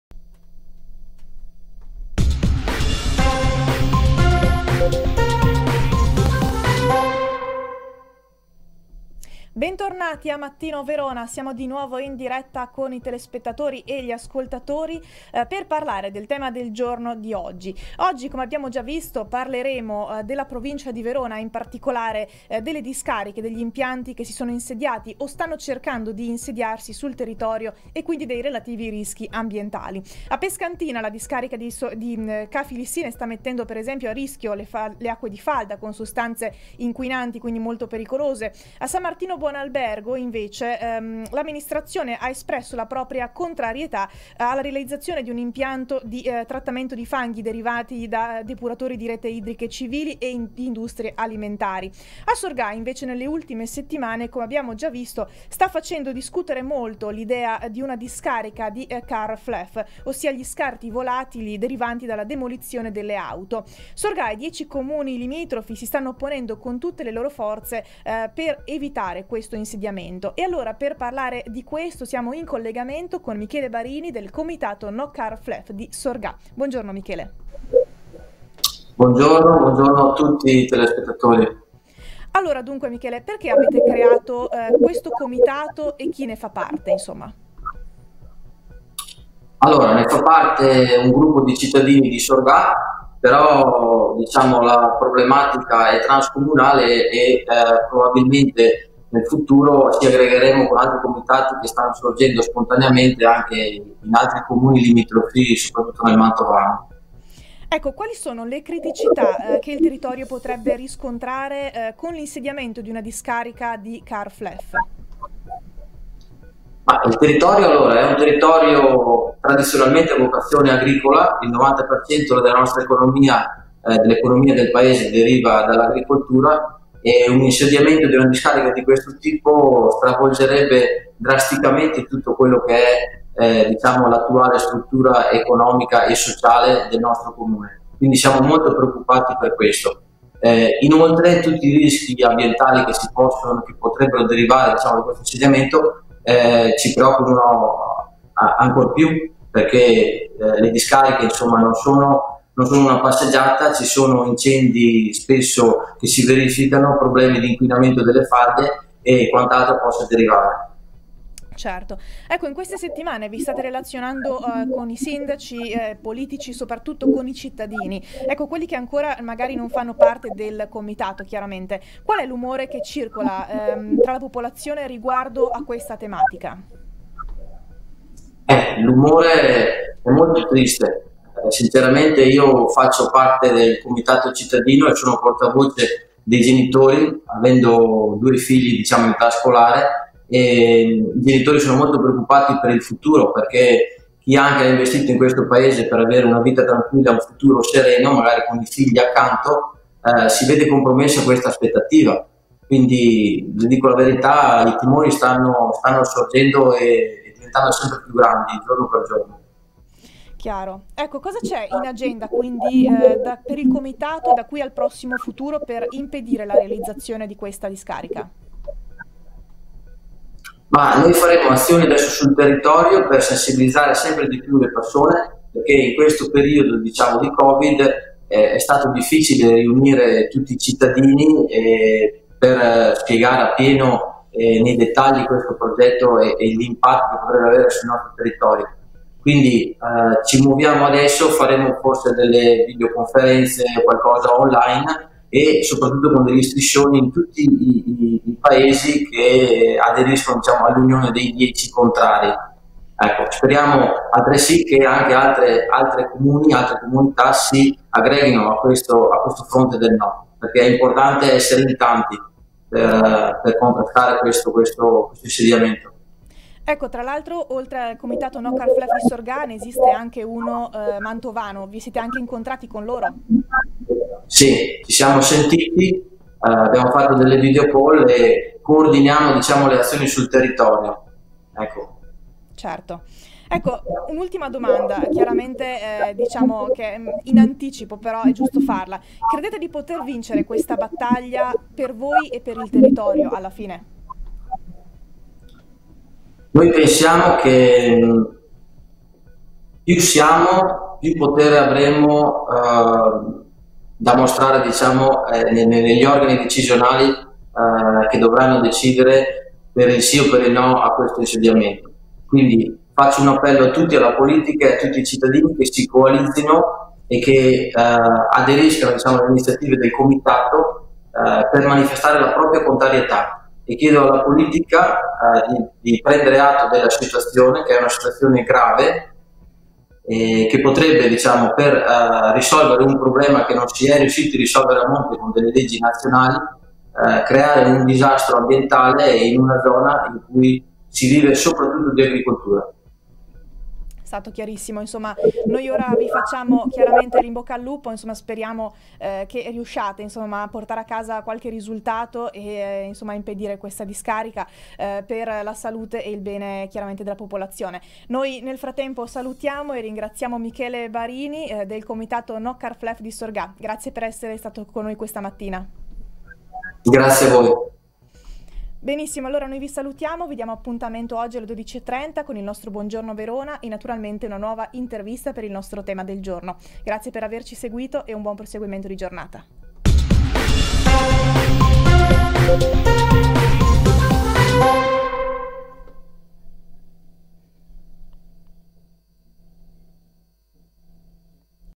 Barini, No Car Fluff Sorgà: «Le discariche non sono una passeggiata»
A parlare delle criticità e delle preoccupazioni dei cittadini in merito al progetto di una discarica car fluff a Sorgà è intervenuto ai microfoni di Radio Adige Michele Barini, del comitato No Car Fluff Sorgà.